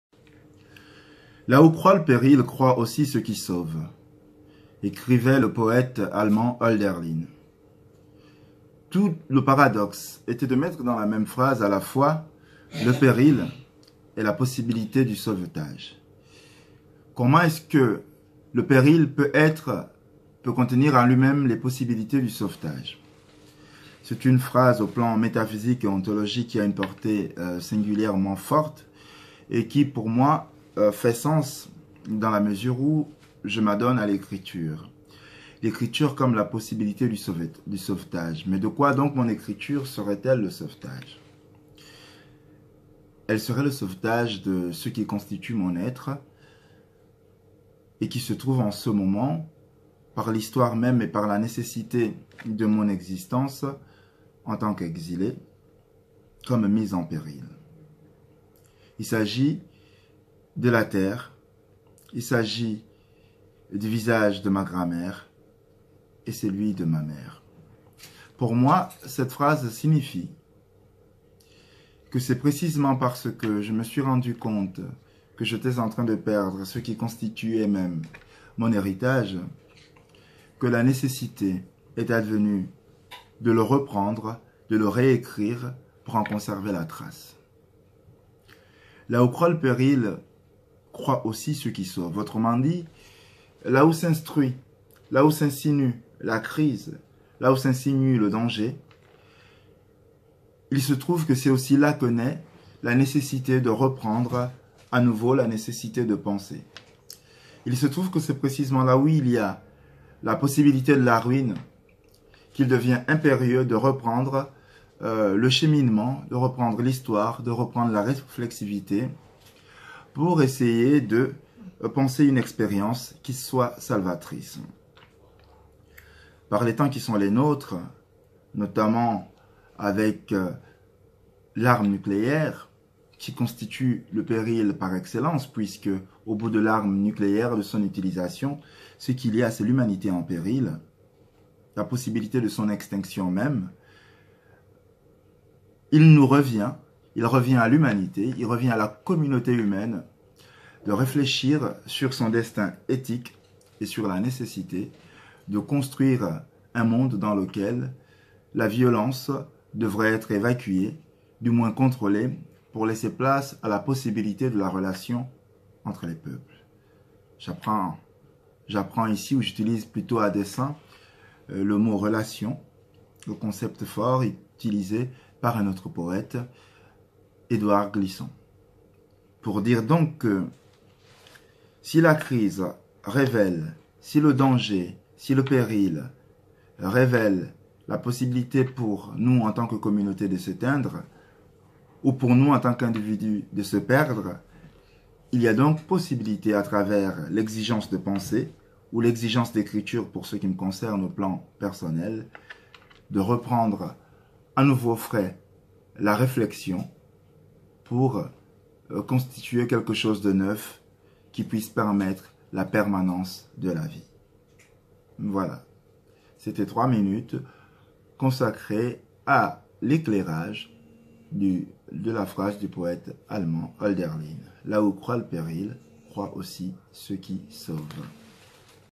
Là où croit le péril, croit aussi ce qui sauve" Hölderlin mouila philosophie philo 3minphilo #
3 minutes pour commenter cette proposition du poète Hölderlin